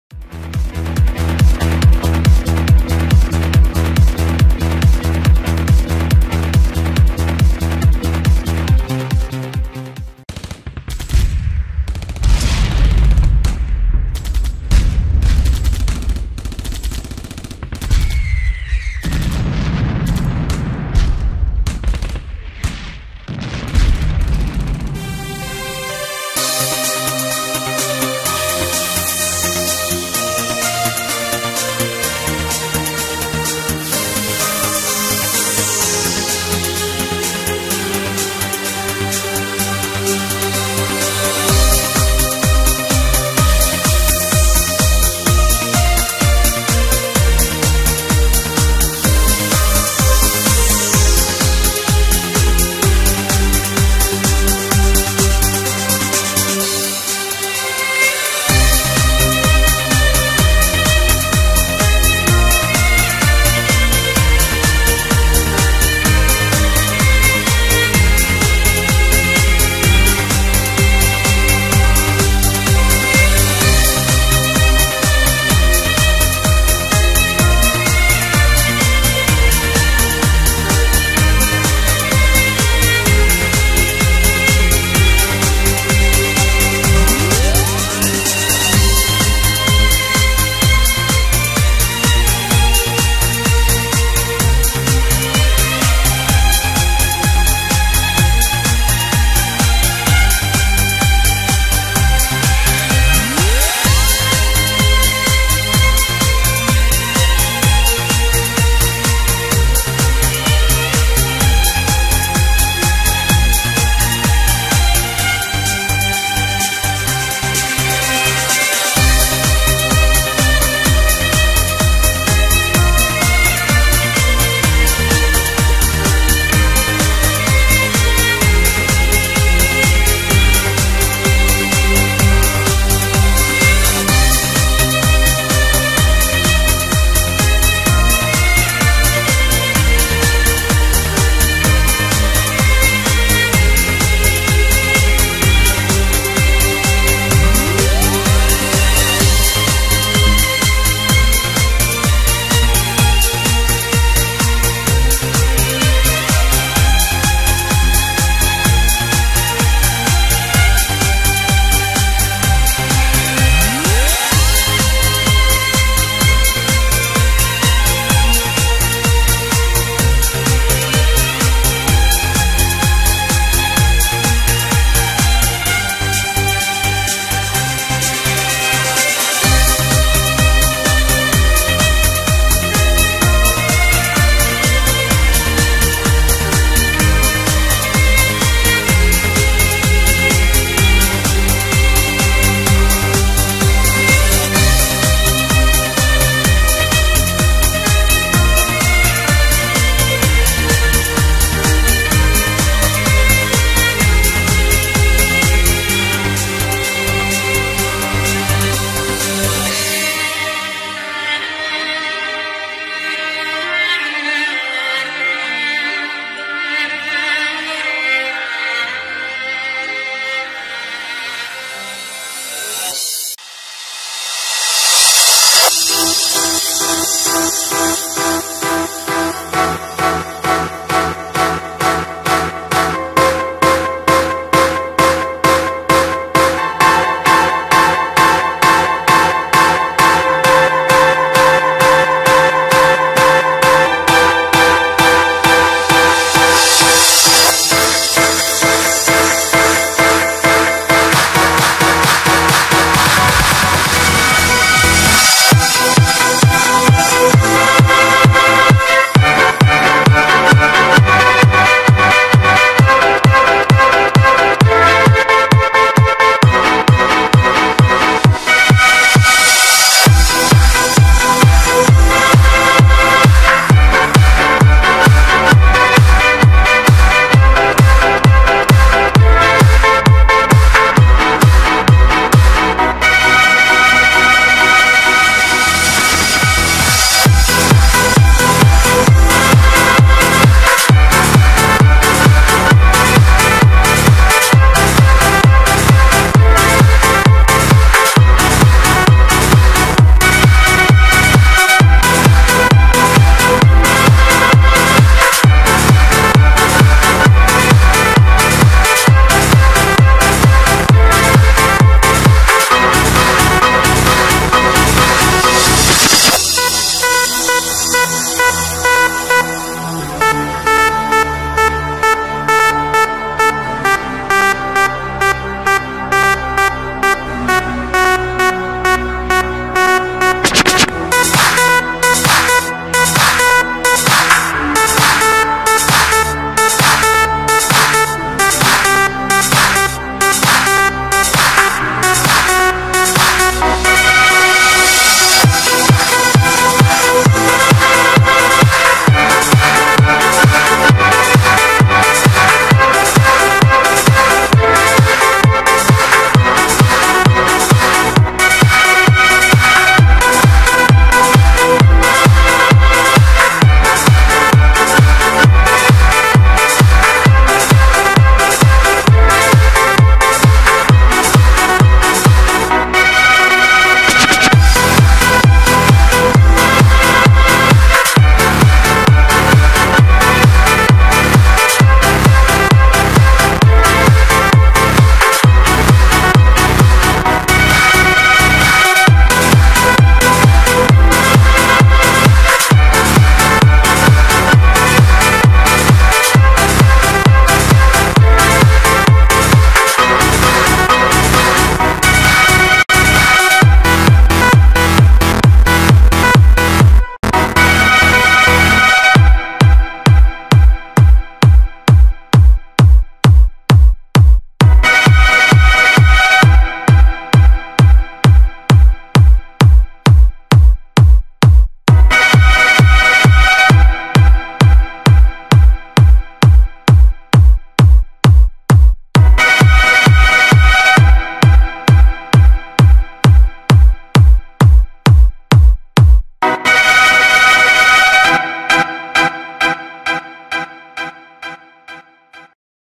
в/ч 3752. 1995 -1996 командировка.
Этот ролик создан из собранных фотографий кто служил в части. в/ч 3752 не только кто был в чечне и проходил службу в части.музыка обработана и вырезана лишнее,спец эффекты сделано мной, высоком качестве  широкой экранном формате.НЕ ДЛЯ РЕКЛАМЫ  а для распространения общественности.